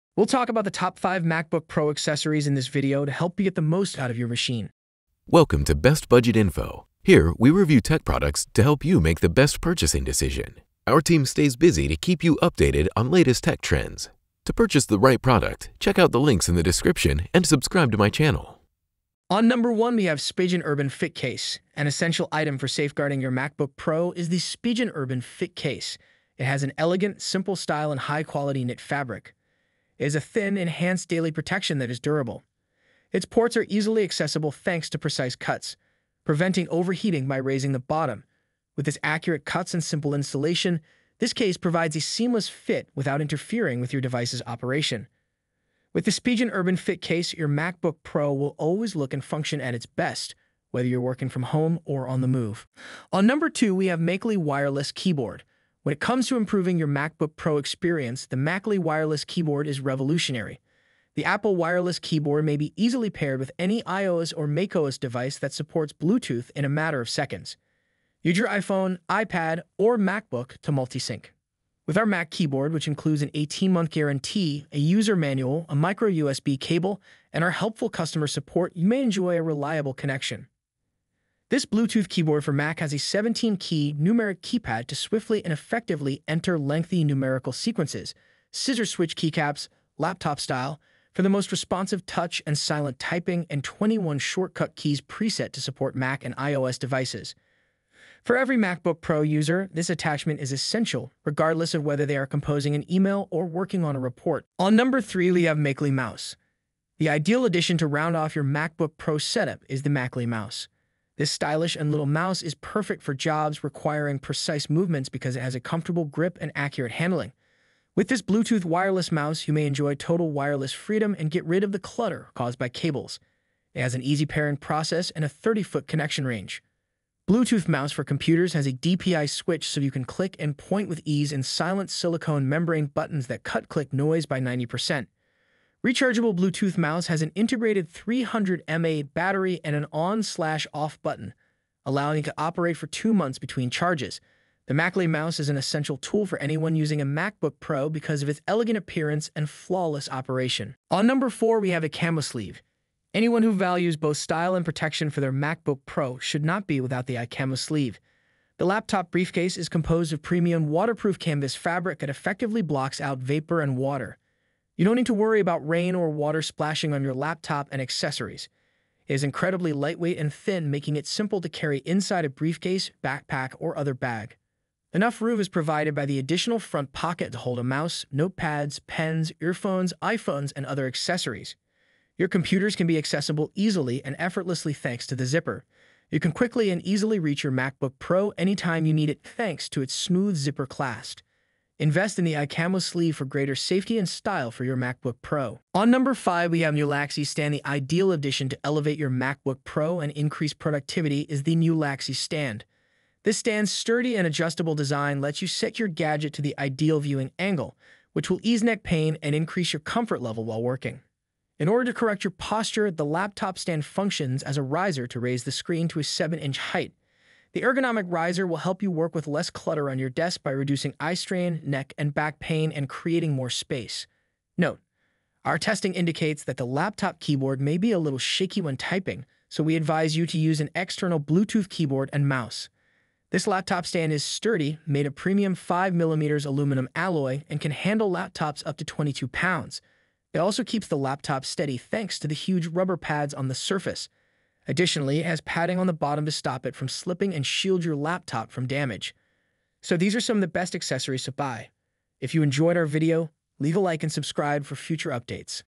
Top 5 Must-Have Accessories for MacBook Pro M3
We'll talk about the top five MacBook Pro 14 and 16 M3 pro and M3 max accessories in this video to help you get the most out of your machine.

1. Spigen urban fit case for MacBook Pro M3

2. Macally wireless keyboard for MacBook Pro M3

3. Macally wireless mouse for the MacBook Pro M3

4. iKammo laptop sleeve for M3 MacBook Pro

5. Nulaxy laptop stand for  MacBook Pro M3 series